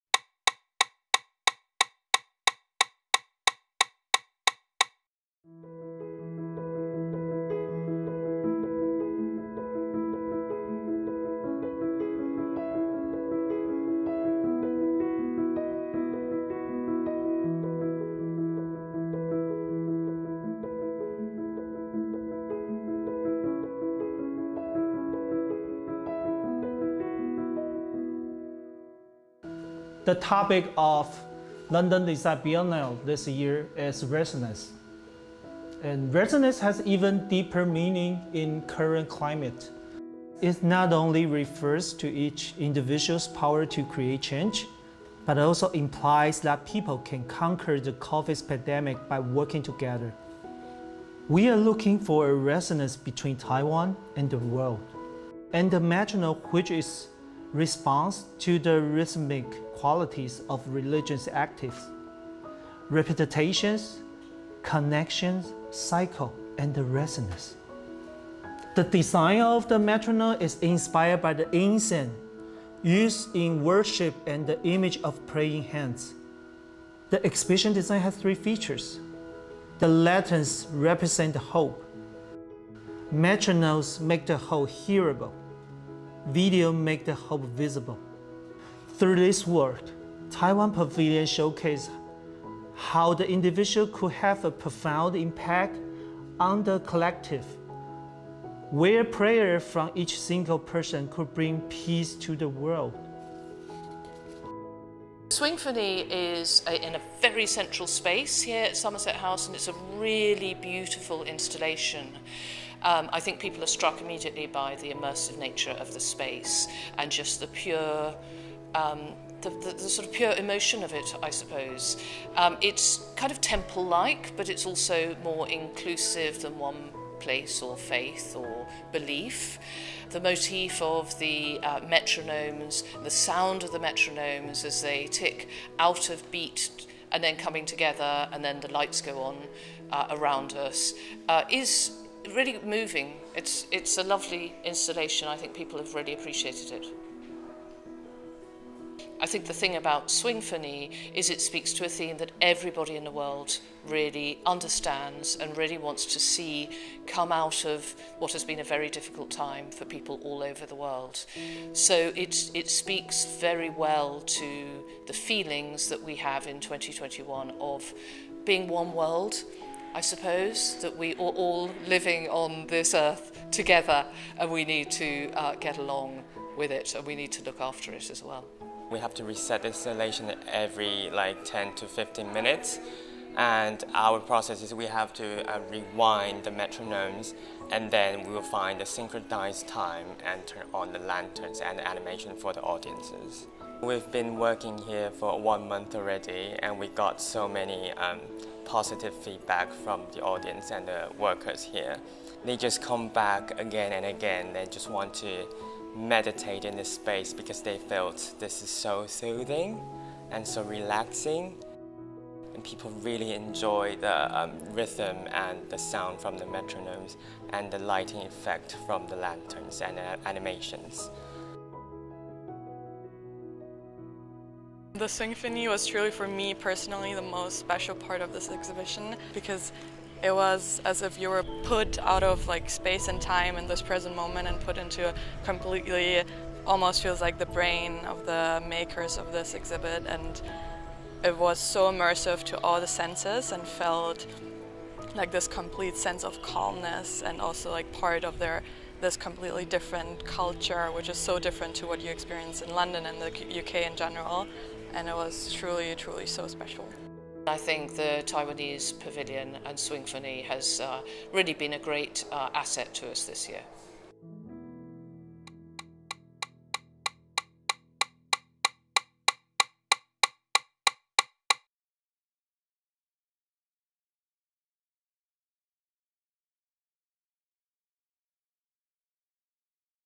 倫敦設計雙年展臺灣館─傳遞臺灣與世界的共振旋律
由文化部支持的倫敦設計雙年展「臺灣館」於2021年6月於倫敦知名古蹟薩默塞特府展出。此次參展由臺灣設計研究院統籌規劃，BITO策展，以「全球同舟一命」議題，推出名為《Swingphony》為主題之展覽。

展覽內容由台灣信仰文化出發，從中萃取出多元、拼接、共融特色的影像設計，並結合科學原理中「共振」的物理現象，企圖呈現個體間各自擺動振盪，在交互影響下，漸漸趨向齊一節奏的現象；延伸演繹台灣在新冠疫情衝擊全球之際，向世界傳達穩定的力量。
Sponsored by the Ministry of Culture, Taiwan, organised by the Taiwan Design Research Institute (TDRI) and curated by Bito, the Taiwan Pavilion presents its Swingphony exhibition at 2021 London Design Biennale.
The Taiwan Pavilion team uses Swingphony as the main concept behind its interpretation of the London Design Biennale’s theme - Resonance. The inspiration comes from the diversity of religions in Taiwan, combined with physics – The Schumann Resonances.

The aim of Swingphony is to enable visitors to understand the natural electromagnetic resonances and experience communal sound rhythms, to highlight the power of unity in a fragmented world.